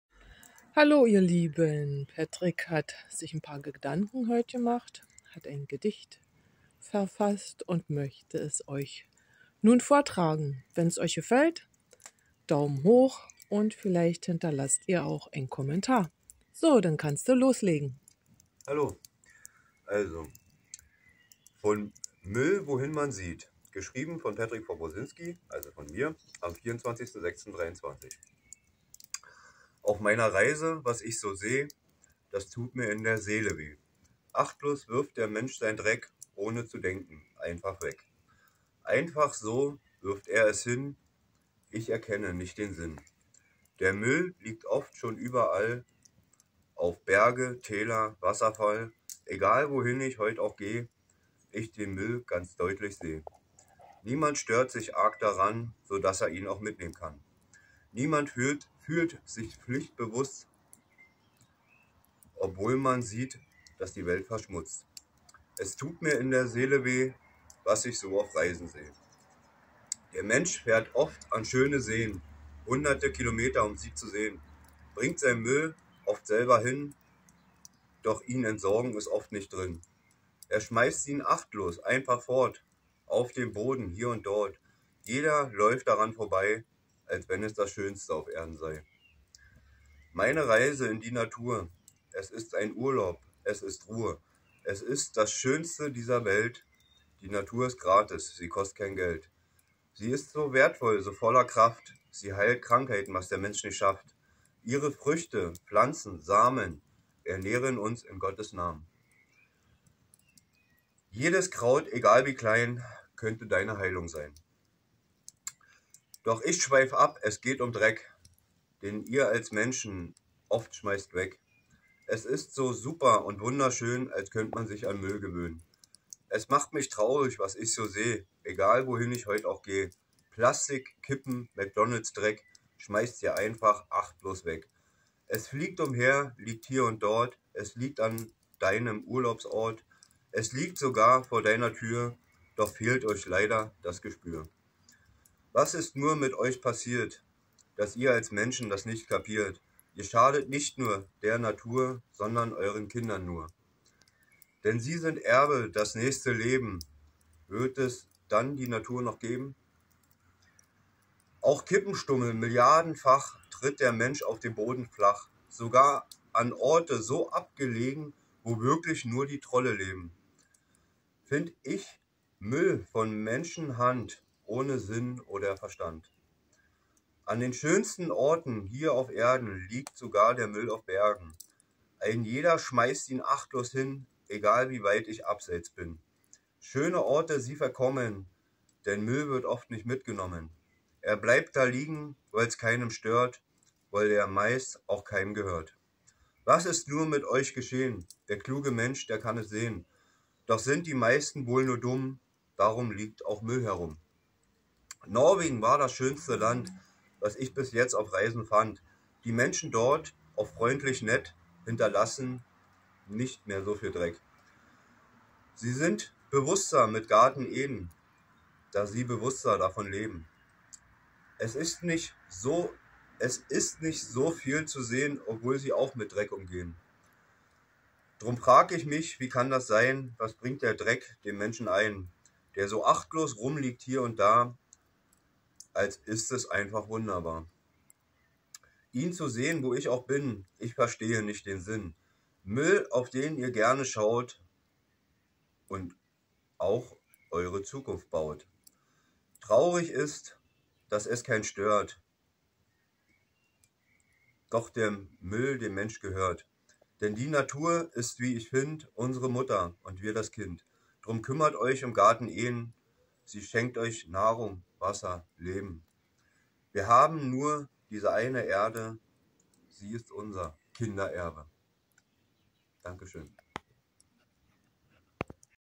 ein Gedicht von mir über unsere Müll Erde 🌍🙏
MÜLL WOHIN MAN SIEHT
Geschrieben von Patrick von Brosinsky
24.6.23

Auf meiner Reise, was ich so seh ,dass tut mir in der Seele weh.
Achtlos wirft der Mensch seinen Dreck, ohne zu denken, einfach weg.
Einfach so wirft er es hin, ich erkenne nicht den Sinn.
Der Müll liegt oft schon überall, auf Berge, Täler, am Wasserfall.
Egal wohin ich heut auch geh, ich den Müll ganz deutlich seh.
Niemand stört sich arg daran, sodass er ihn auch mitnehmen kann.
Niemand fühlt sich flichtbewusst, obwohl man sieht, dass die Welt verschmutzt.
Es tut mir in der Seele weh,
was ich so auf der Reise seh.

Der Mensch fährt oft an schöne Seen,
hunderte Kilometer, um sie zu sehen.
Bringt sein Müll oft selber hin, doch ihn entsorgen, ist oft nicht drin.
Er schmeißt ihn achtlos einfach fort,
auf den Boden, hier und dort.
Jeder läuft daran vorbei, als wenn es das Schönste auf Erden sei .

Meine Reise in die Natur, es ist ein Urlaub, es ist Ruhe, es ist das Schönste dieser Welt. Die Natur ist gratis, sie kostet kein Geld .
Sie ist so wertvoll, so voller Kraft,
sie heilt Krankheiten, was der Mensch nicht schafft.
Ihre Früchte, Pflanzen, Samen, ernähren uns in Gottes Namen.
Jedes Kraut egal wie klein, könnte deine Heilung sein.🌿

Doch ich schweif ab, es geht um Dreck, den ihr als Menschen oft schmeißt weg.
Es ist so super und wunderschön, als könnt man sich an Müll gewöh'n.
Es macht mich traurig, was ich so seh,
egal wohin ich heut auch geh.
Plastik, Kippen, Mc Donald's, Dreck, schmeißt ihr einfach achtlos weg .
Es fliegt umher, liegt hier und dort, es liegt an deinem Urlaubsort.
Es liegt sogar vor deiner Tür, doch fehlt euch leider das Gespühr.
Was ist nur mit euch passiert?
Das ihr als Menschen, dass nicht kapiert?
Ihr schadet nicht nur die Natur, sondern euren Kindern nur .
Denn sie sind Erbe, dass nächste Leben, wird es dann die Natur noch geben?
Auch Kippenstummel milliardenfach, tritt der Mensch auf dem Boden flach.
Sogar an Orte so abgelegen, wo wirklich nur die Trolle leben .
Find ich Müll von Menschenhand, ohne Sinn, oder Verstand.

An den schönsten Orten, hier auf Erden, liegt sogar der Müll auf Bergen.
Ein Jeder schmeißt ihn achtlos hin,
egal wie weit ich abseits bin .
Schöne Orte sie verkommen, denn Müll wird oft nicht mitgenommen.
Er bleibt da liegen, weil's keinem stört,
weil er meist auch keinem gehört .
Was ist nur mit euch geschehen?
Der kluge Mensch, er kann sehen,
doch sind die Meisten wohl nur dumm.
Darum liegt auch Müll herum.

Norwegen war das schönste Land, was ich bis jetzt, auf Reisen fand.
Die Menschen dort, oft freundlich, nett, hinterlassen auch nicht soviel Dreck.
Sie sind bewusster, mit Garten Eden,
da sie bewusst auch davon leben.
Es ist nicht so viel zu sehen,
obwohl sie auch mit Dreck umgehen.

Drum frag ich mich, wie kann das sein?
Was bringt der Dreck, dem Menschen ein?
Der so achtlos rumliegt, hier und da,  als ist es einfach wunderbar.
Ihn zu sehen, wo ich auch bin, ich verstehe nicht den Sinn .
Müll, auf den ihr gerne schaut und auch eure Zukunft baut .
Traurig ist, dass es keinen stört, doch der Müll, dem Mensch gehört.

Denn die Natur ist wie ich find,
unsere Mutter und wir das Kind.
Drum kümmert euch, um Garten Eden
sie schenkt euch Nahrung, Wasser, Leben.

Wir haben nur diese eine Erde, 🌍🙏
sie ist unser Kinder Erbe .